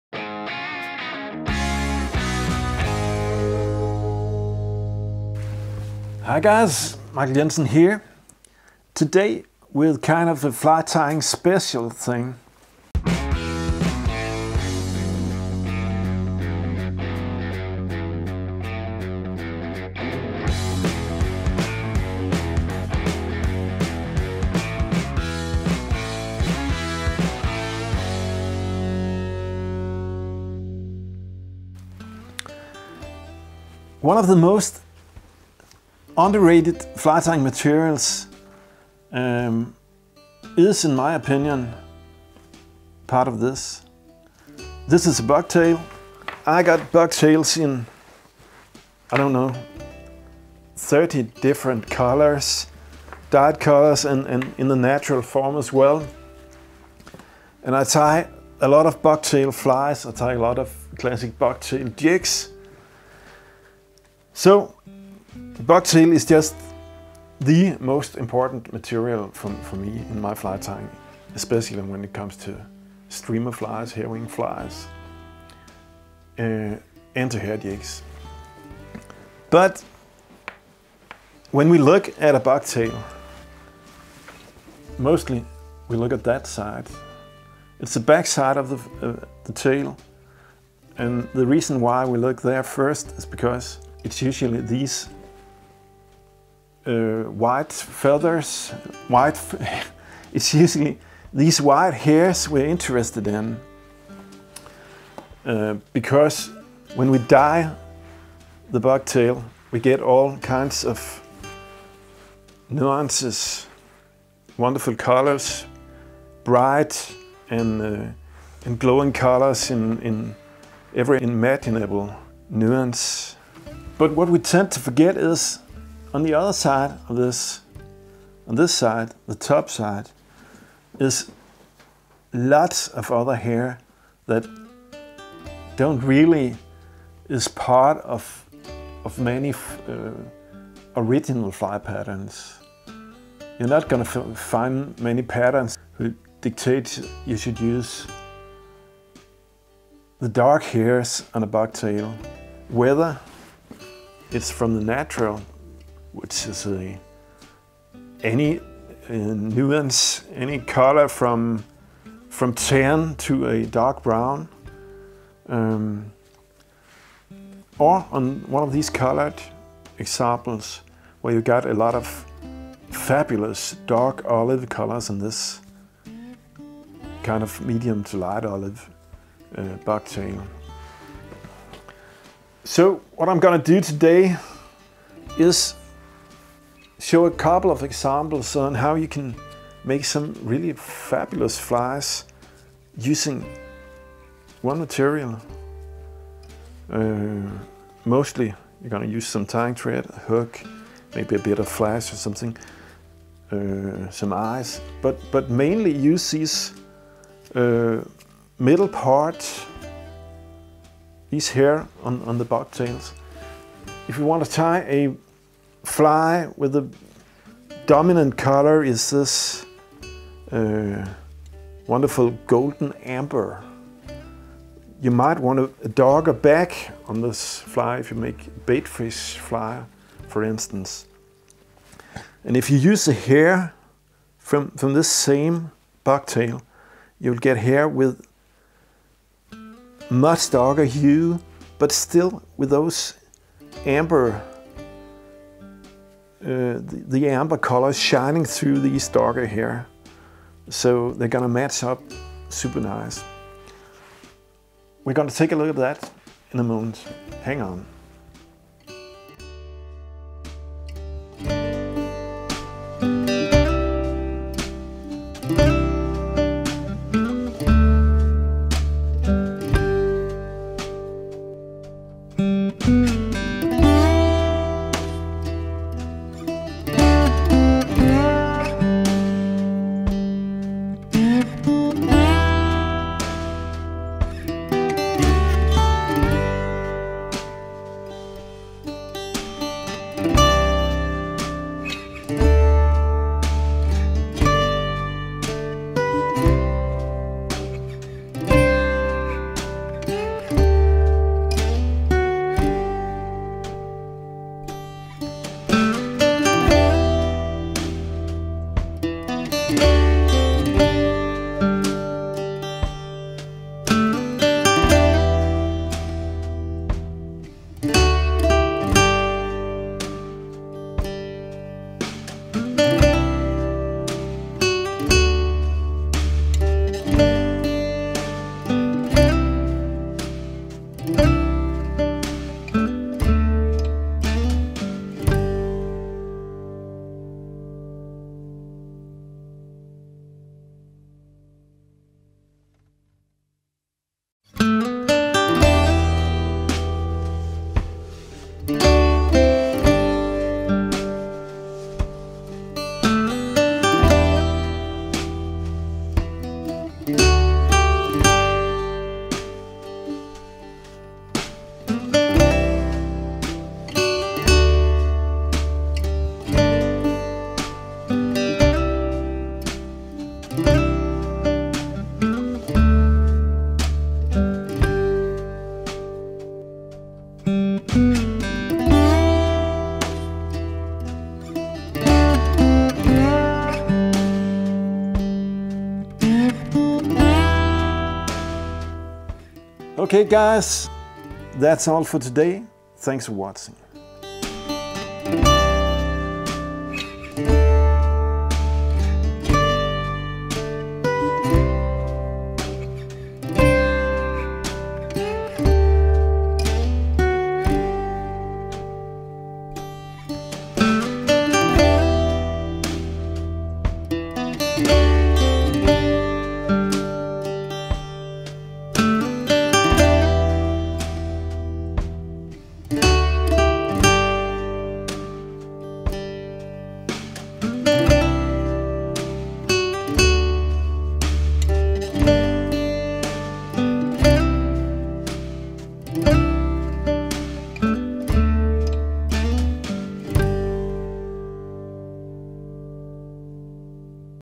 Bucktail - turning to the dark side - fly tying special
Bucktail is a fabulous material for fly tying and jig tying. But most of us probably primarily use the long hairs on the backside of the bucktail. In this video I give a couple of reasons why you should use the darker hair on the material. These hairs are underrated, but have a lot of potential. And… besides… they are there for the taking - and account for even more bang for your bucks.

This video includes a couple of examples on flies tied with that philosophy in mind. These are only shown in speeded tying sequences, but are now available as seperate videos on our channel right here:

Michael and Ulla

Books:
Streamers & Bucktails, Joseph D. Bates Jr.
Thunder Creek Flies, Keith Fulsher
Tying & Fishing Bucktails, Mike Valla
Hair-Hackle Tying, Gordon Mackenzie

You wanna see the camera gear and microphones I use for making my YouTube videos?